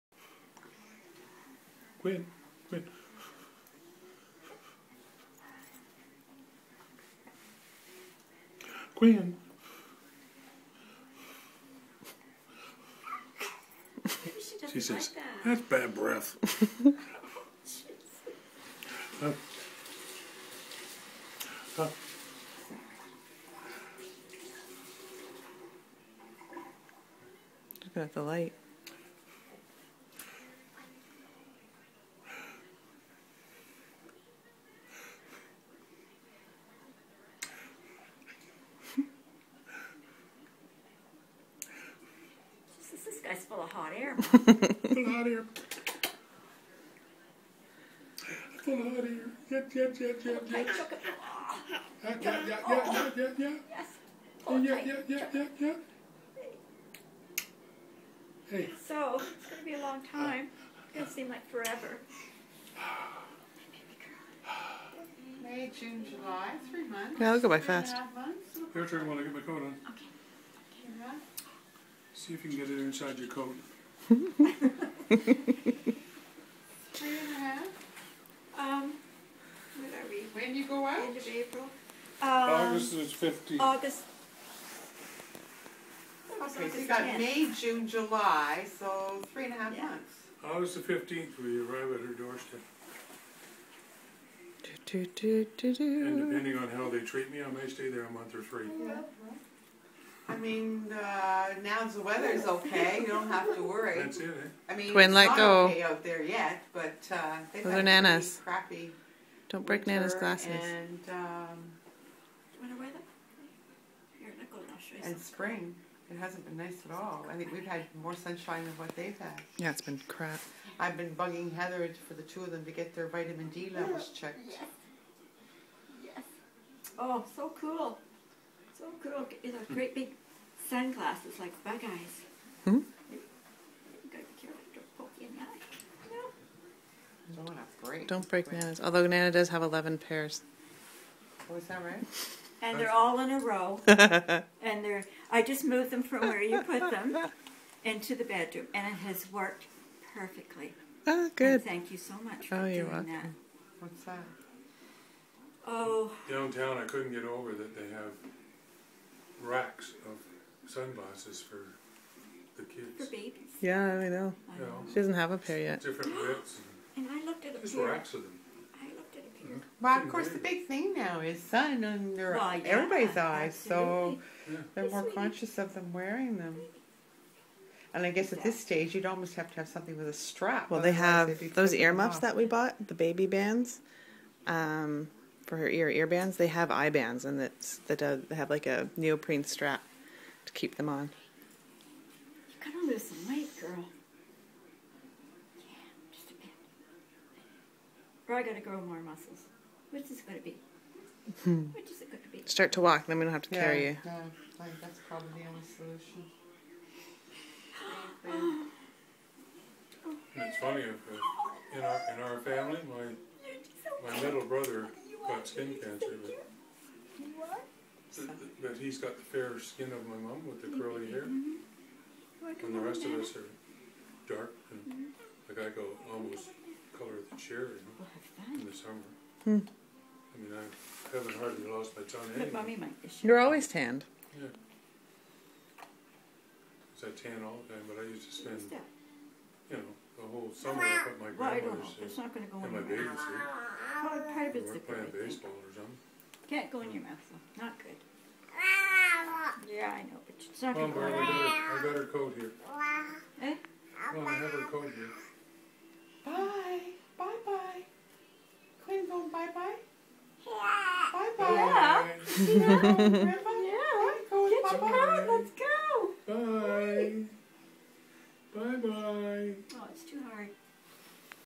Quinn with Nana & Papa!!
Quinn loving Nana & Papa and Nana & Papa loving Quinn!